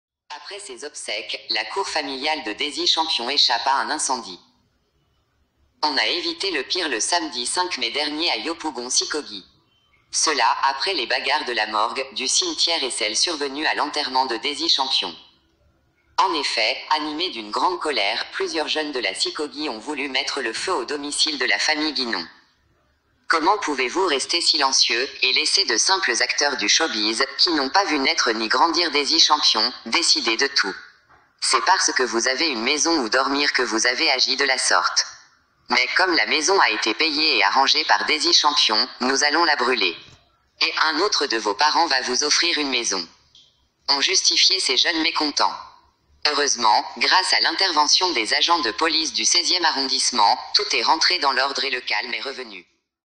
Après ses obsèques, la cour familiale de Dezy Champion échappe à un incendie On a évité le pire
Après ses obsèques, la cour familiale de Dezy Champion échappe à un incendie

On a évité le pire le samedi 05 mai dernier à Yopougon Sicogi. Cela, après les bagarres de la morgue, du cimetière et celles survenues à l’enterrement de Dezy Champion. En effet, animés d’une grande colère, plusieurs jeunes de la Sicogi ont voulu mettre le feu au domicile de la famille Guinon.

« Comment pouvez-vous rester silencieux, et laisser de simples acteurs du showbiz, qui n’ont pas vu naître ni grandir Dezy Champion, décider de tout ?
C’est parce que vous avez une maison où dormir que vous avez agi de la sorte. Mais, comme la maison a été payée et arrangée par Dezy Champion, nous allons la brûler. Et, un autre de vos parents va vous offrir une maison ». Ont justifié ces jeunes mécontents.

Heureusement, grâce à l’intervention des agents de police du 16ème arrondissement, tout est rentré dans l’ordre et le calme est revenu.